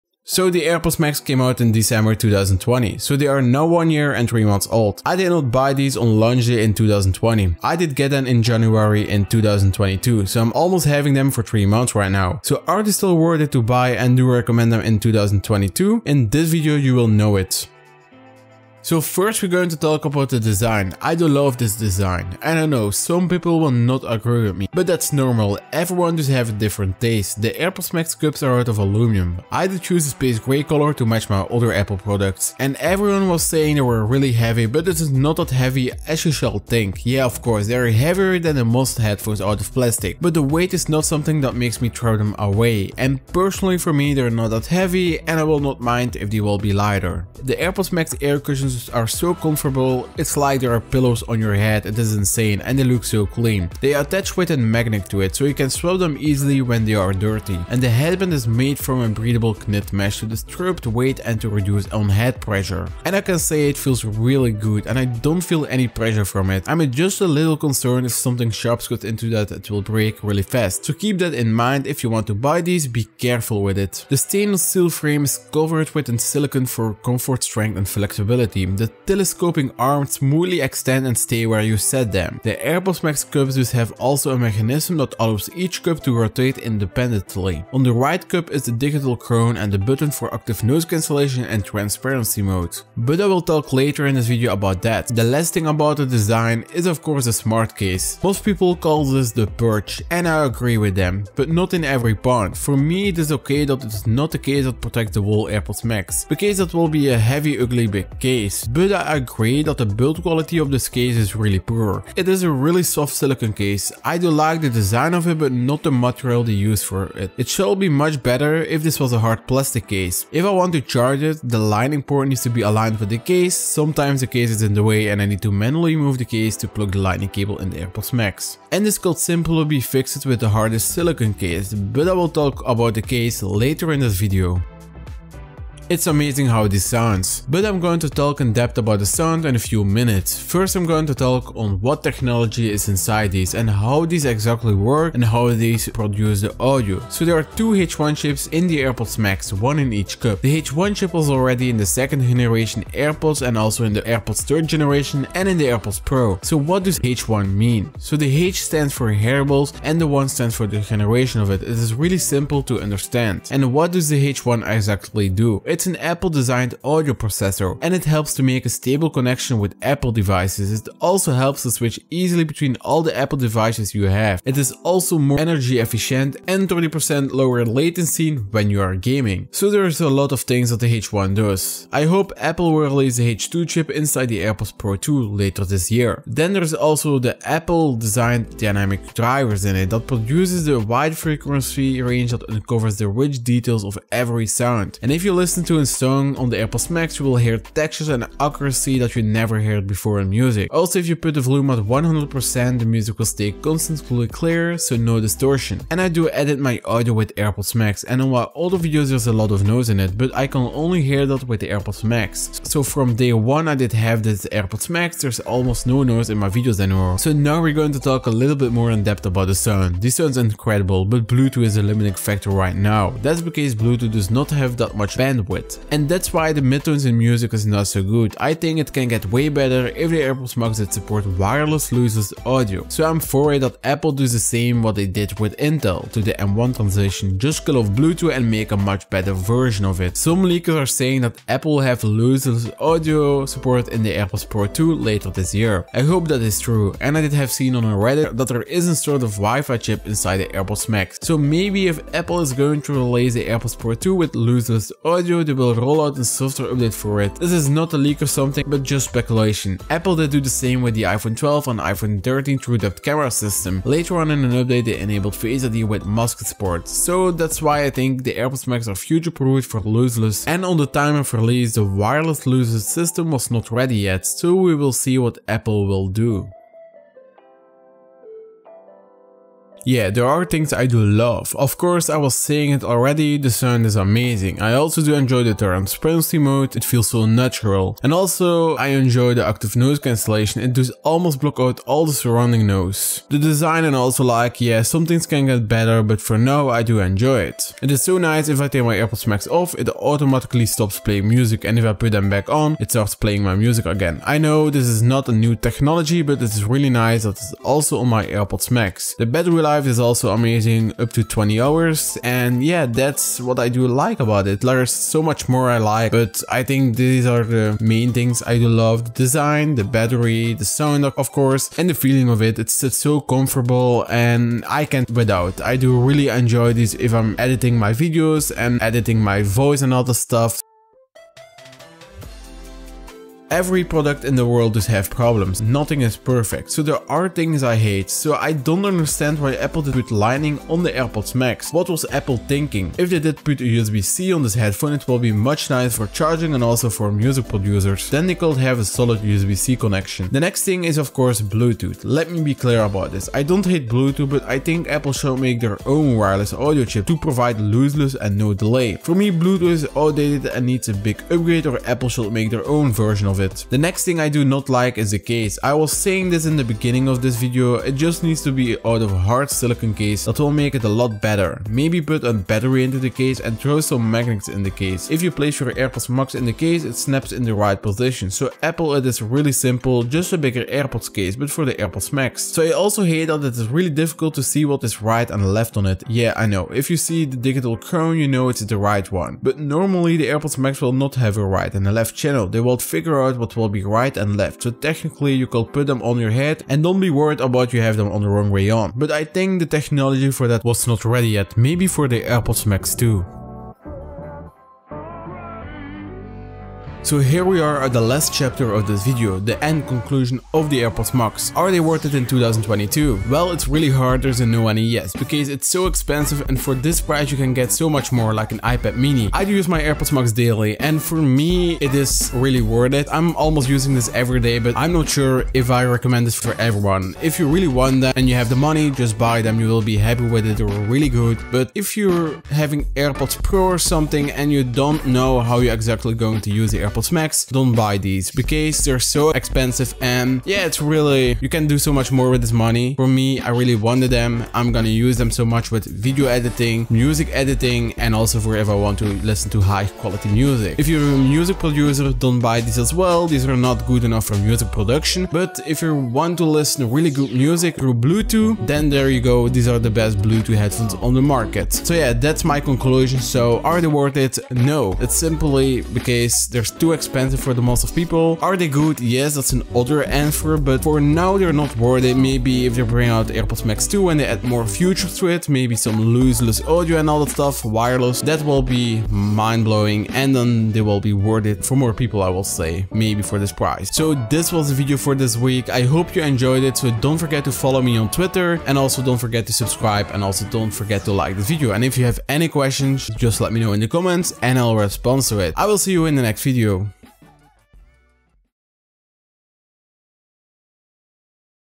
Are the Airpods max still worth it in 2022? (4k)
Welcome back to a new video on my channel.
are the AirPods max still worth it in 2022?
in this video you will know the answer

And on Instagram -  @ReVinTonTech

    *Chapters*
00:00 Intro
00:15 The Design
2:18 The Sound
4:39 What do I like?
6:44 The conclusion
8:04 Closing the video